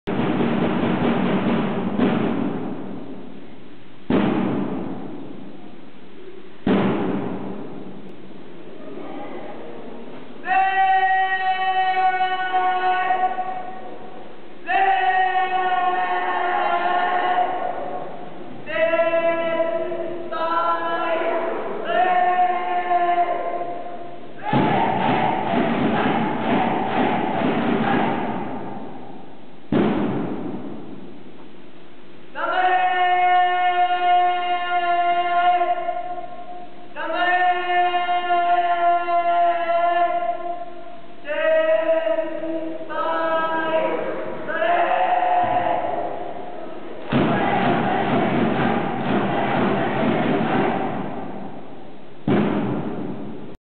Ue-Chu 1st years Cheer for the 3rd years
This was taken at the celebration for the 3rd years who are graduating this year from my JHS.   So cute... they are saying "Senpai! Ganbare!"